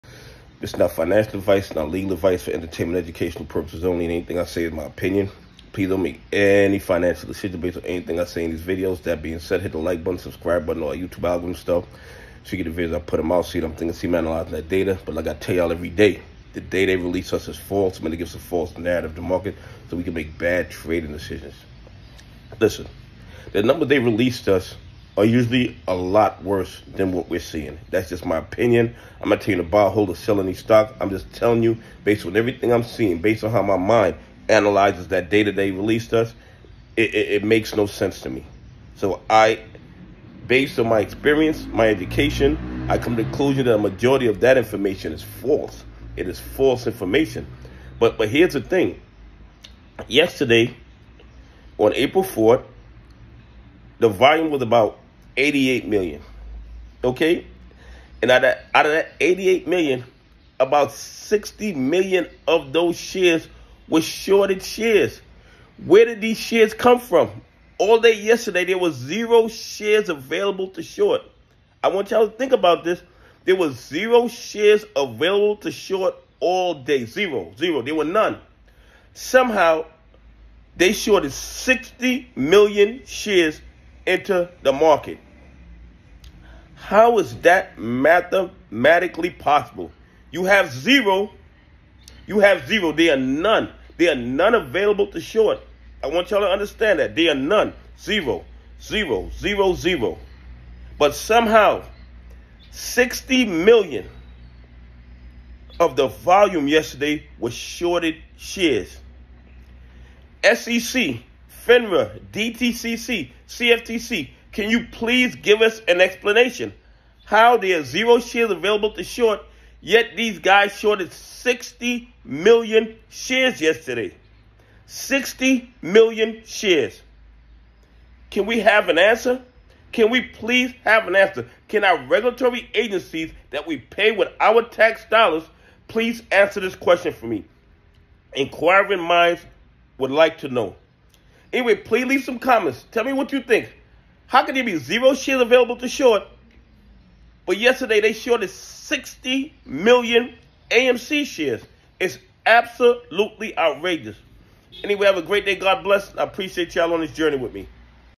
0 AMC shares available to short on 4/4 but 60,000,000 shares were shorted Criminality in real time
How do you have 0 shares available to short all day but somehow 60 million of the volume was shorted shares Can someone explain this to me